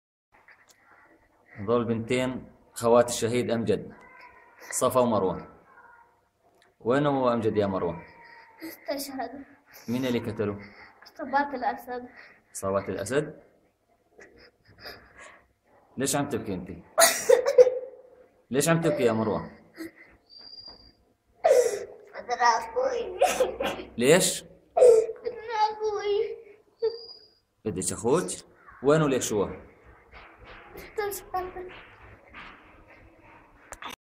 Iran's loyal dog Al-Assad killed the brother of these two girls in Da'el town in Syria 30.10.2011
Iran's loyal dog Bashar Al-Assad killed the brother of these two girls (Amjad Hamid Al-A'asimi) in Da'el town in Daraa province in Syria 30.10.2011